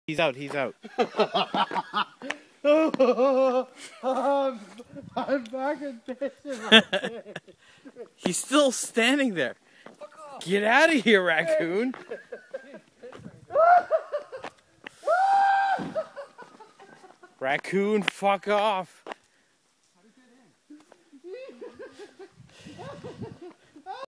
Raccoon in car
A raccoon got into my friends' car and hilarity insued!